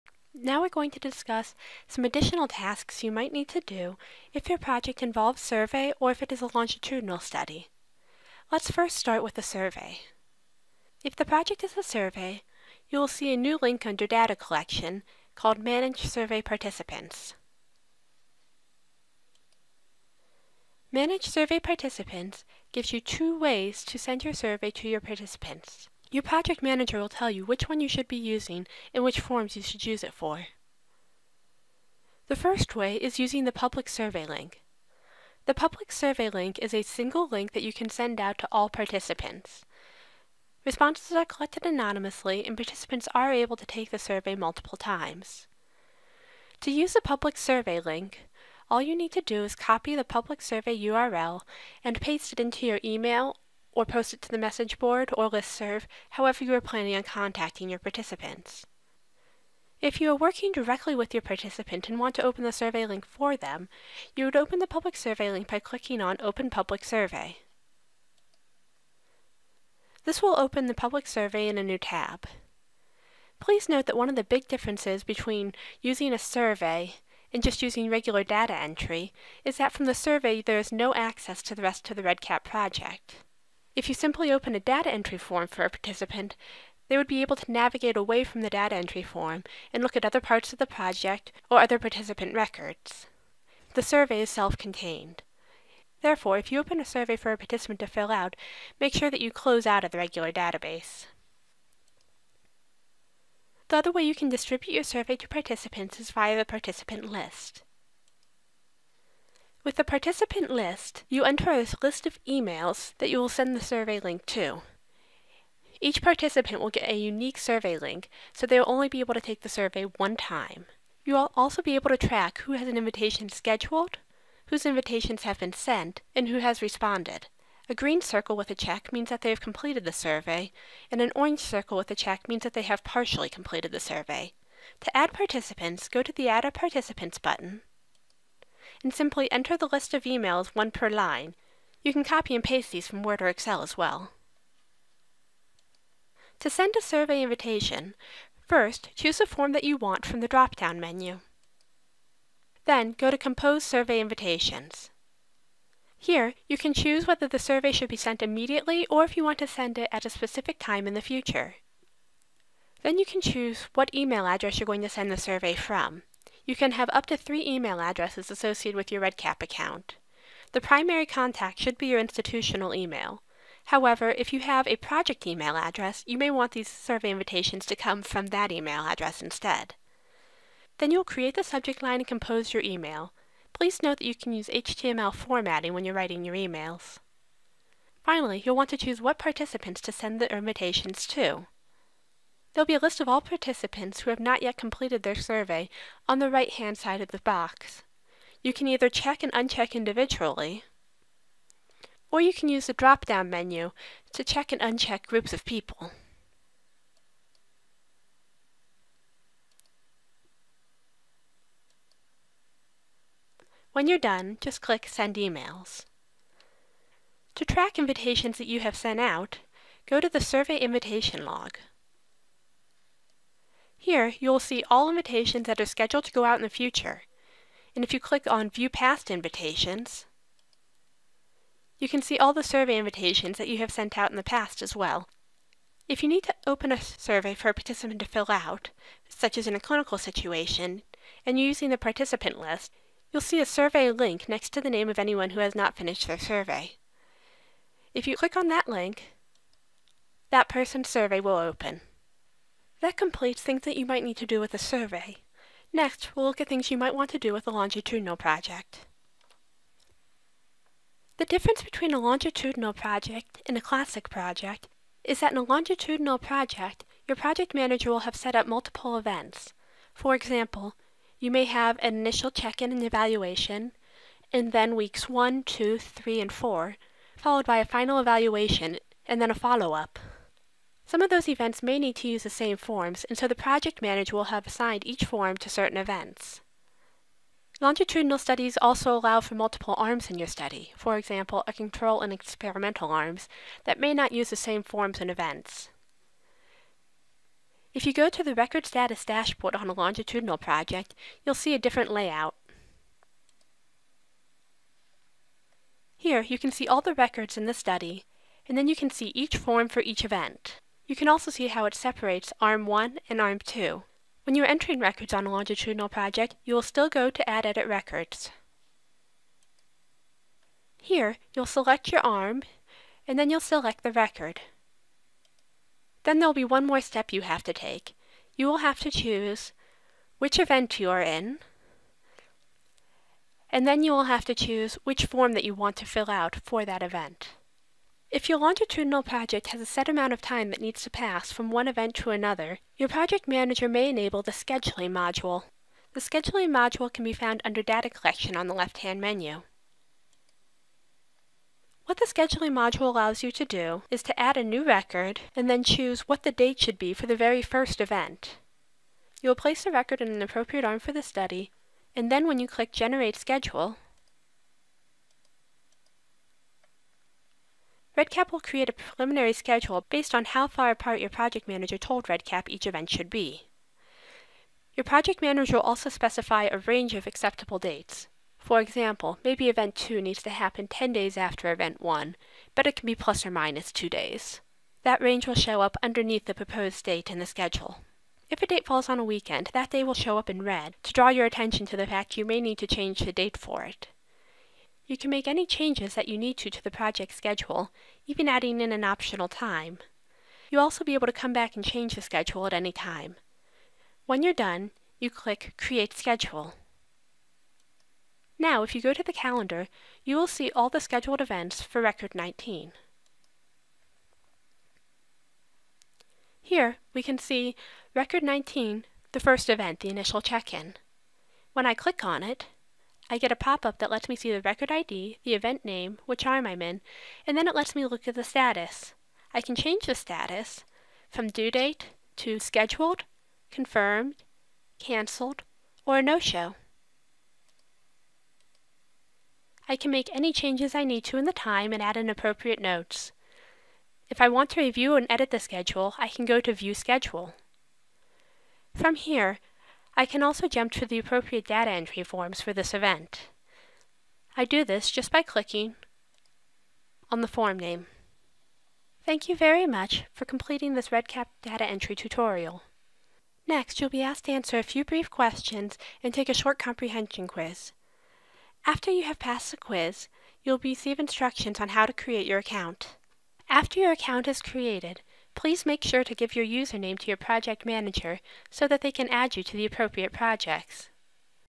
4- REDCap Surveys and Longitudinal Projects for Data Entry Accounts
This is part four of the University of Colorado REDCap tutorial for basic (data-entry) accounts.